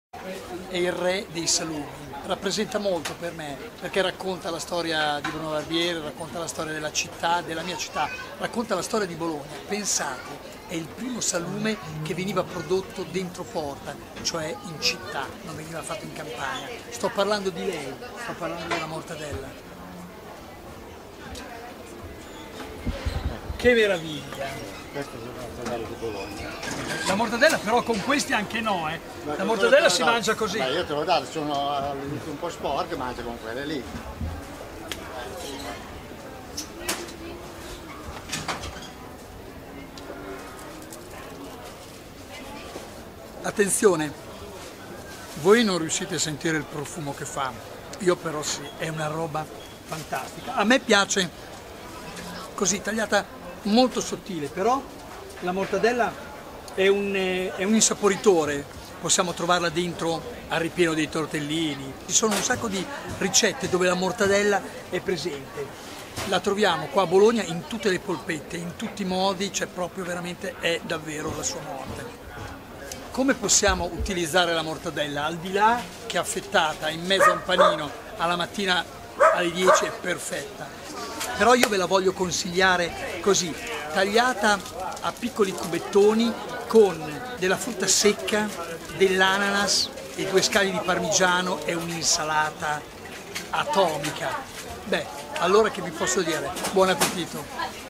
Mortadella: come utilizzare il re dei salumi | Bruno Barbieri Chef
Un salume che amo moltissimo, legato alla mia storia e alle mie origini... la mortadella è un vero e proprio simbolo di Bologna. È il primo salume prodotto dentro le porte della città, e non in campagna. Io la adoro così, affettata sottile, ma è anche un ottimo insaporitore, ad esempio nel ripieno dei tortellini o nelle polpette. Ma come possiamo utilizzarla?

FINALMENTE ONLINE IL MIO E-SHOP! Potete visitarlo cliccando qui